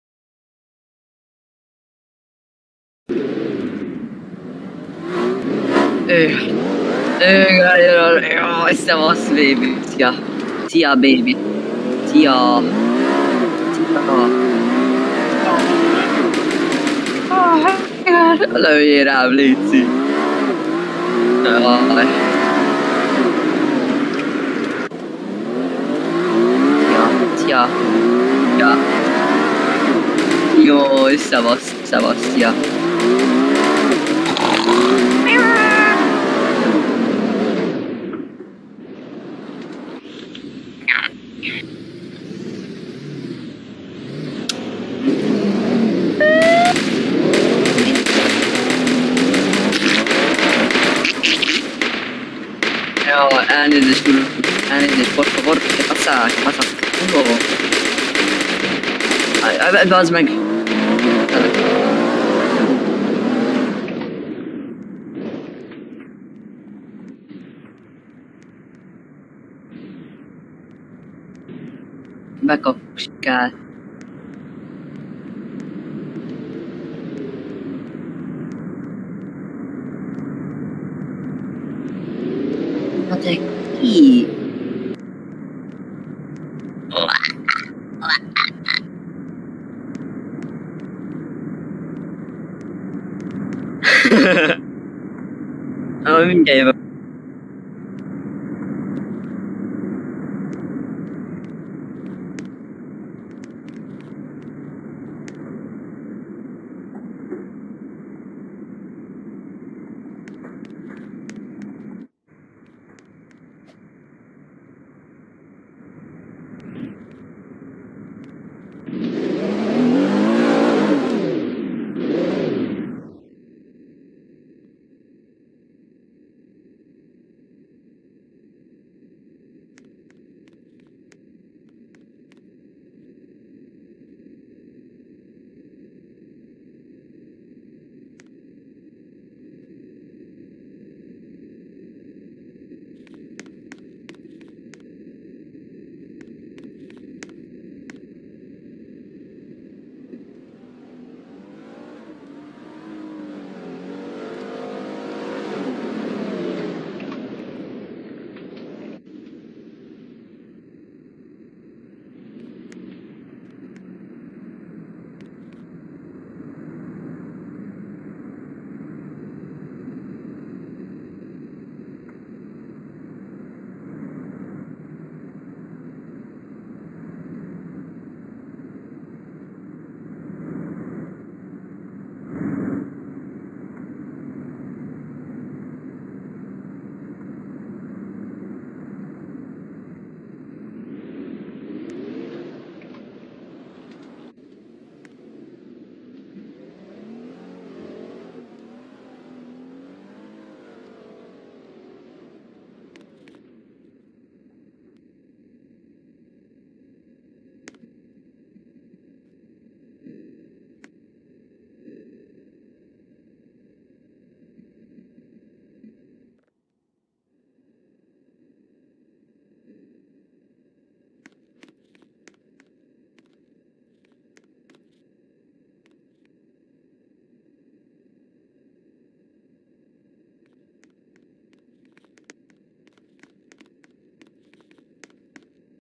Hogyan intézik a Kecskemét rendőri az incidenseket?
Ebben a videóban láthassák, hogy mennyire is biztonságos a játék, és az életveszélyes incidenseket hogyan intézik el a szerepjátékon belül!

A Kecskeméti Rendőrség hozta a színvonalát! Nem csalódtunk benne!